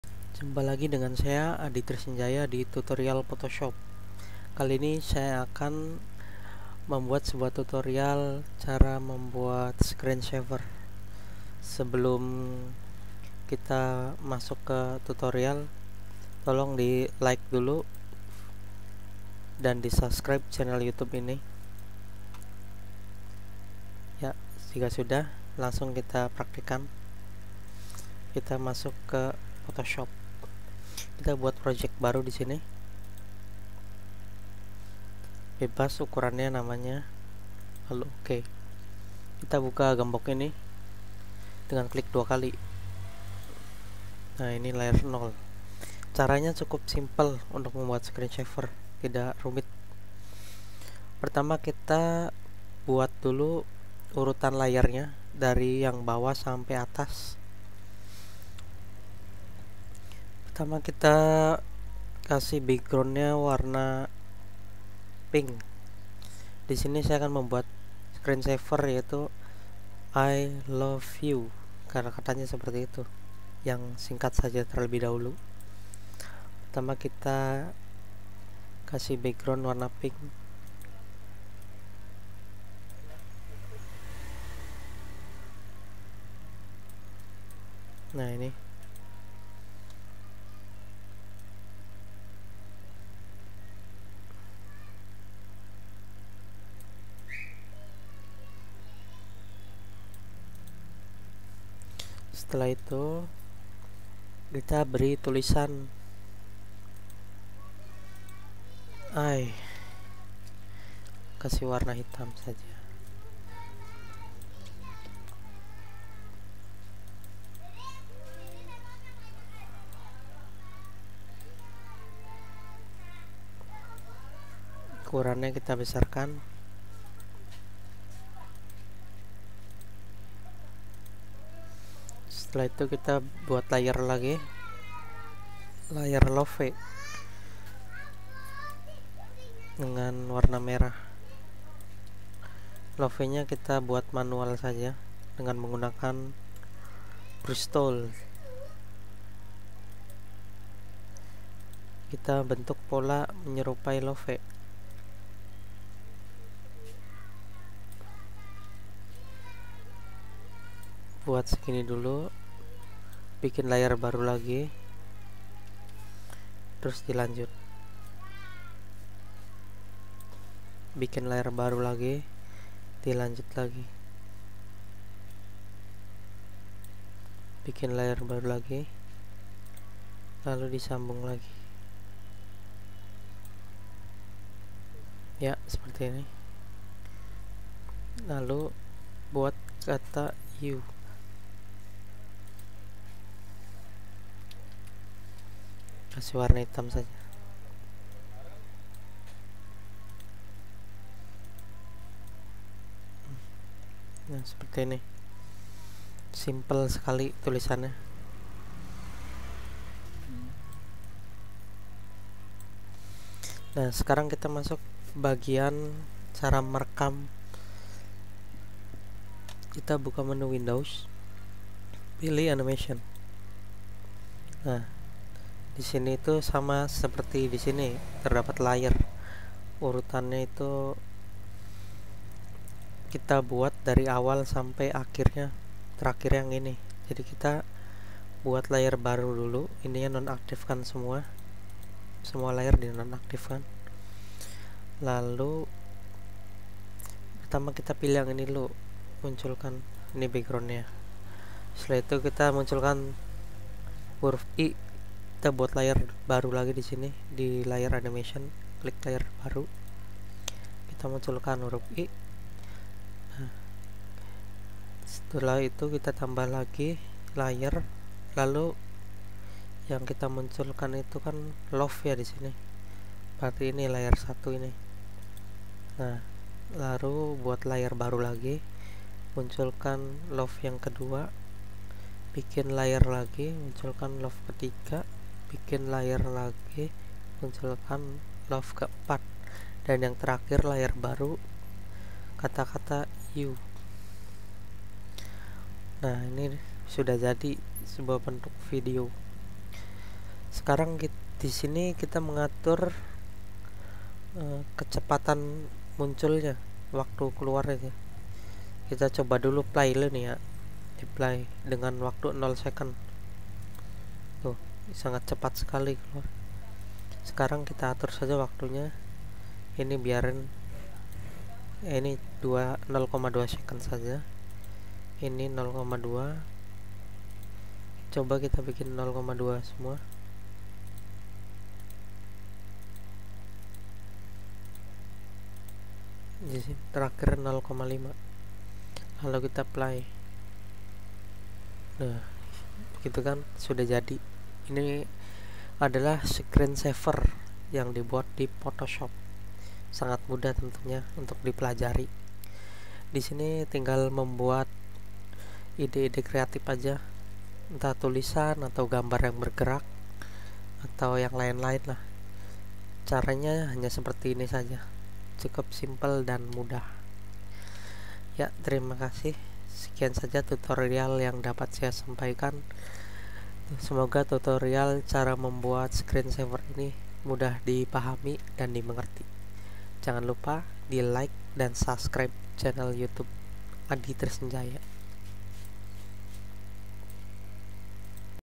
Membuat scrensaver dengan Photoshop
Cara membuat Screensaver sederhana bertuliskan I Love U